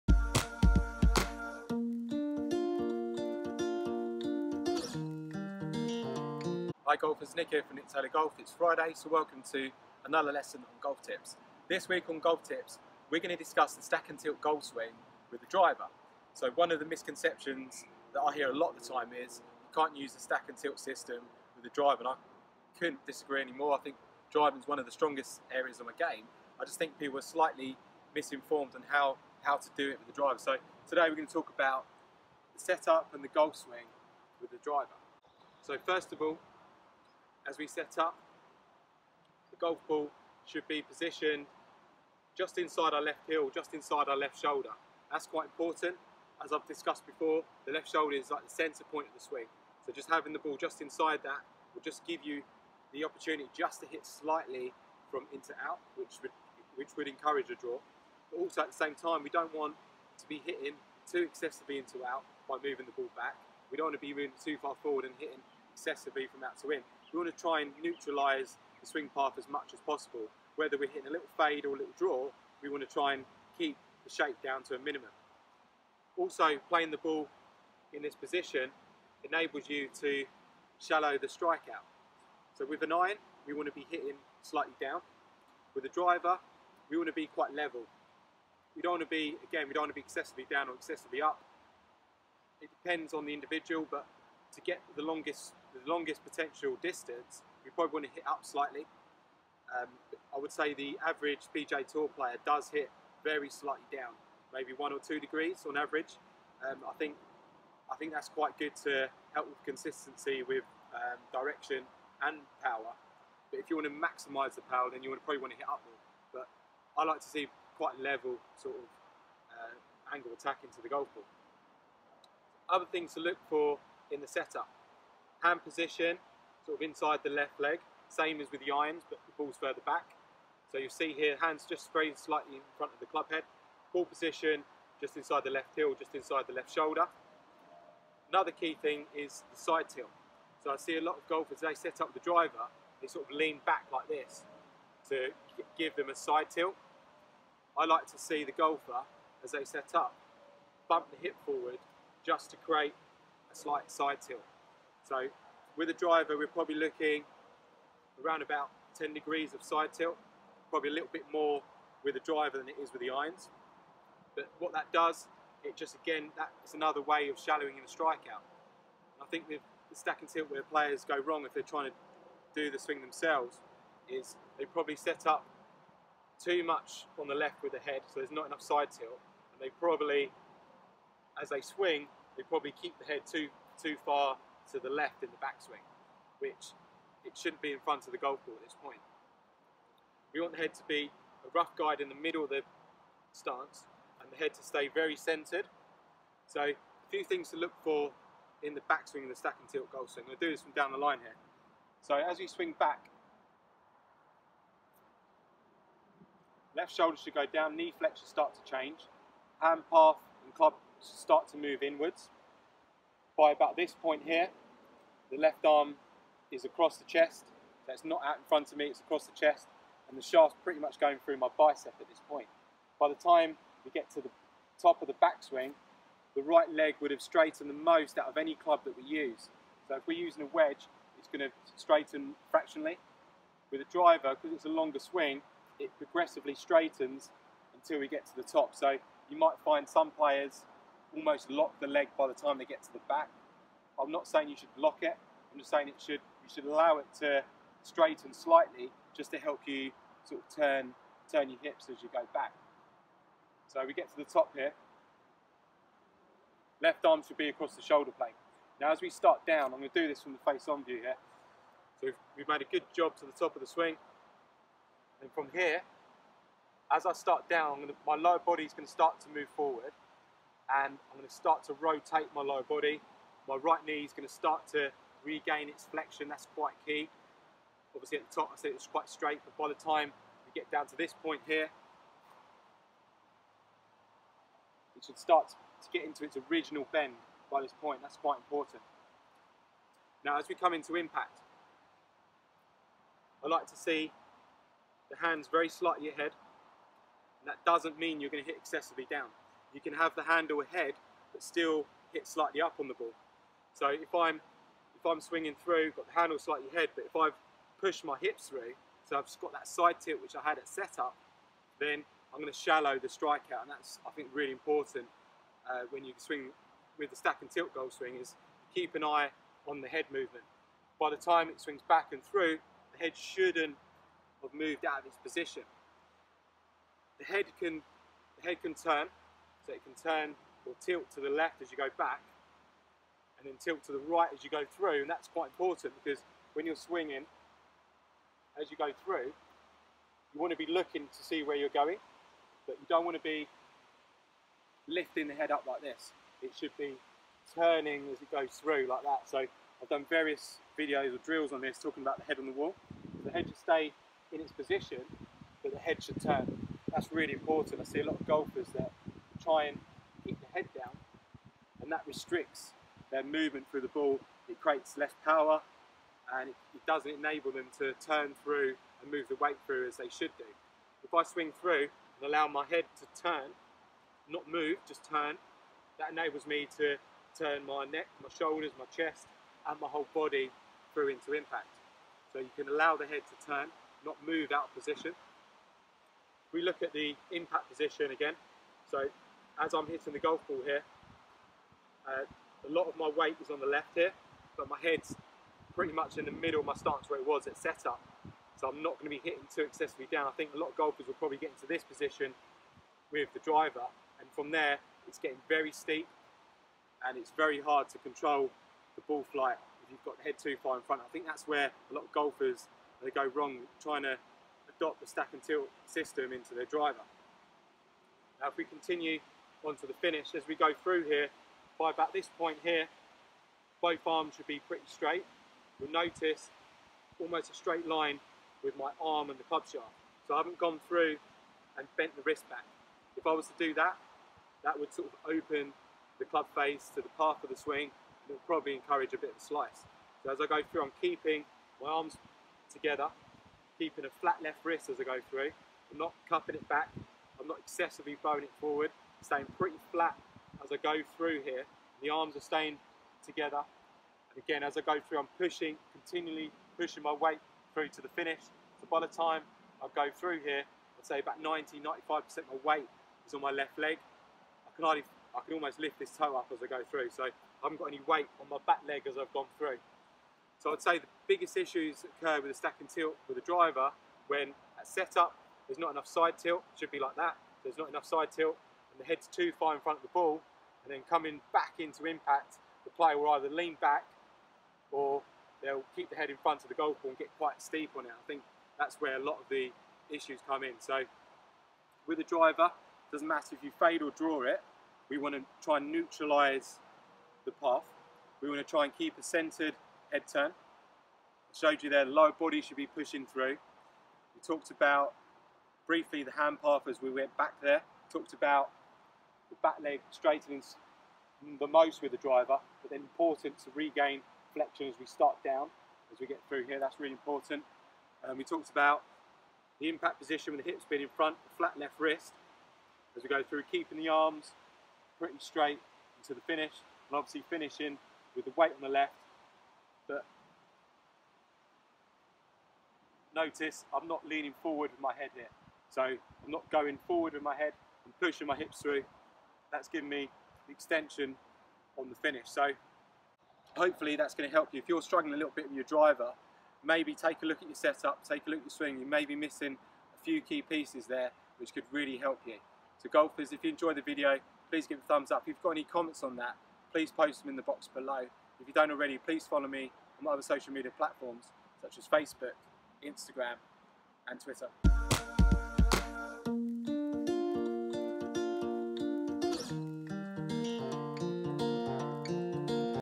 Stack & Tilt with the driver | Golf Tips | Lesson 53
In this lesson of golf tips, PGA professional and Stack and Tilt instructor Nick Taylor discusses the driver swing for stack and tilt. A lot of golfers struggle to adopt the stack and tilt golf swing with the driver. Hopefully this video will those golfers understand the Stack and tilt golf swing for the driver.

FRIDAYS - GOLF TIPS